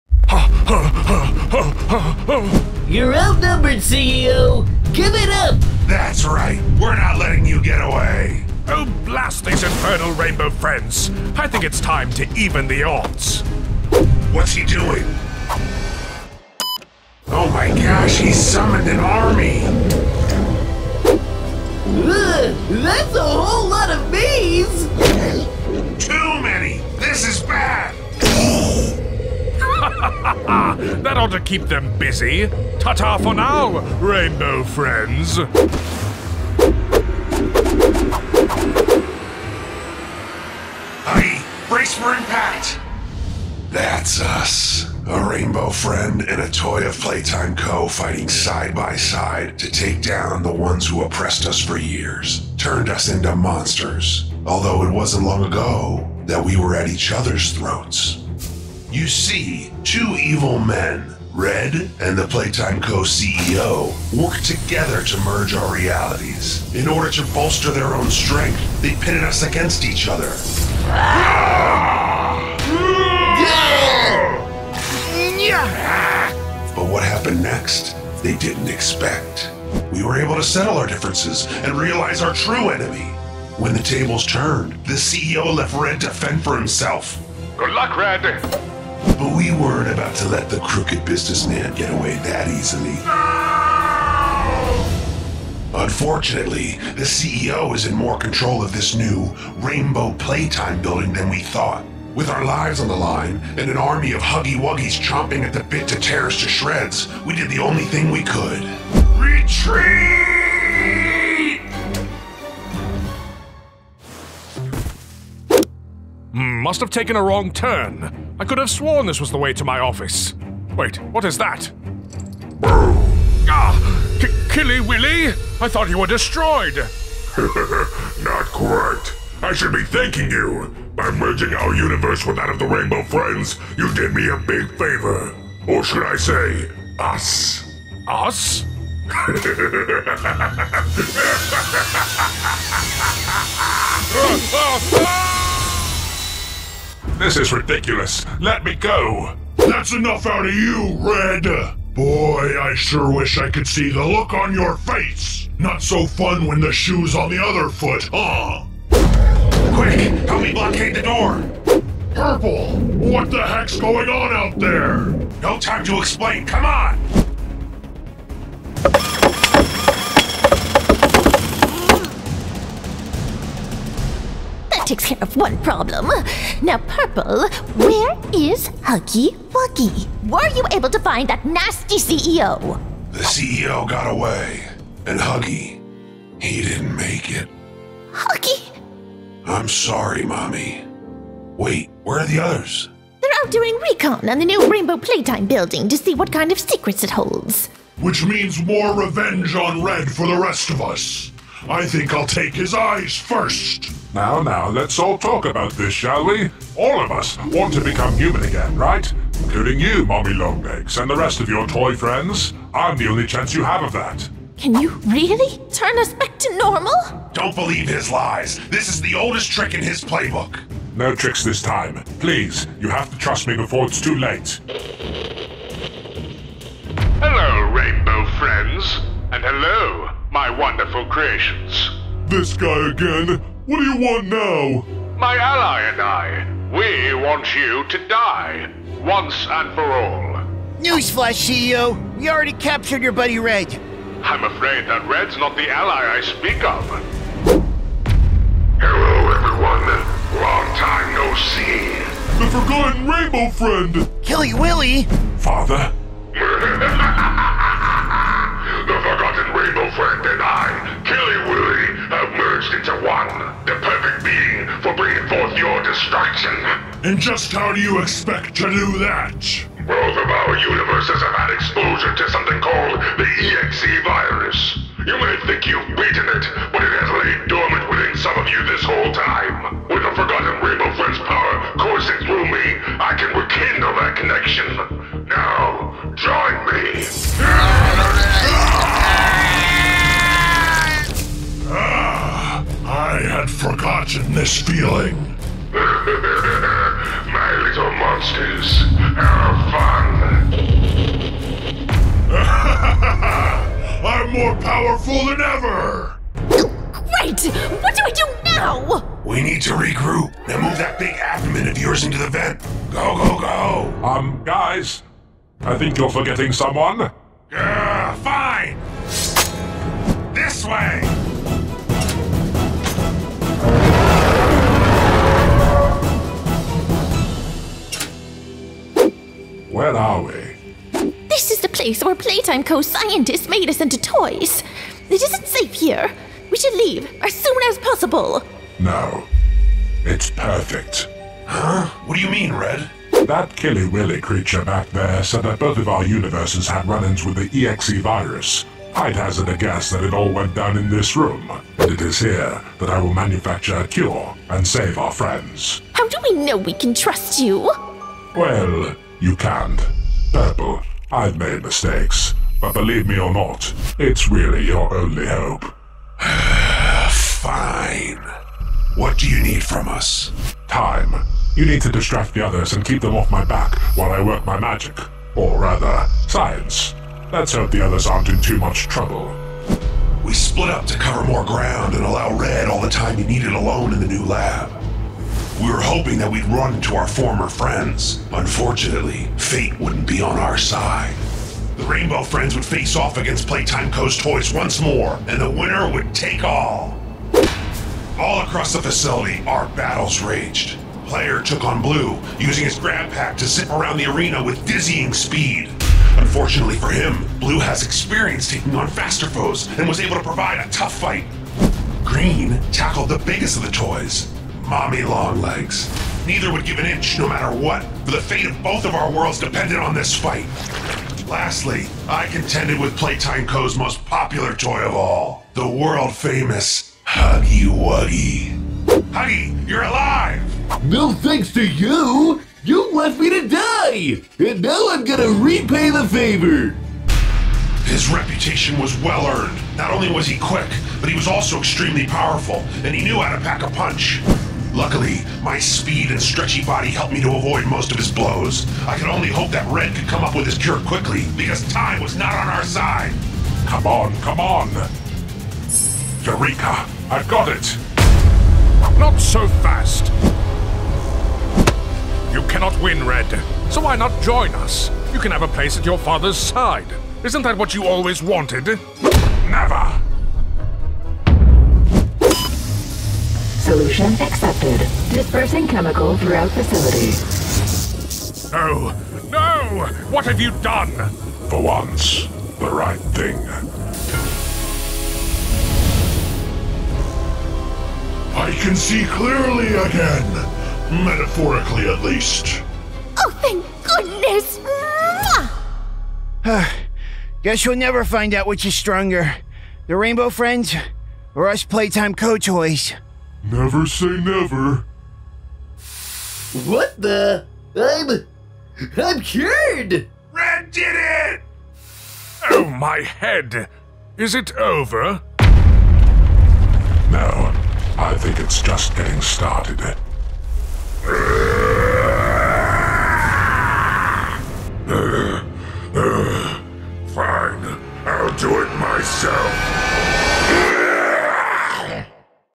PROJECT: PLAYTIME vs. RAINBOW FRIENDS?! (Cartoon Animation)
The Play Place pals of Red, Blue, Green, Orange and Purple all end up in another head to head fight against the monsters of Poppy Playtime, Player, Poppy, Huggy Wuggy, Mommy Long Legs and the CEO. But to make matters worse, an evil lurking below the surface has returned...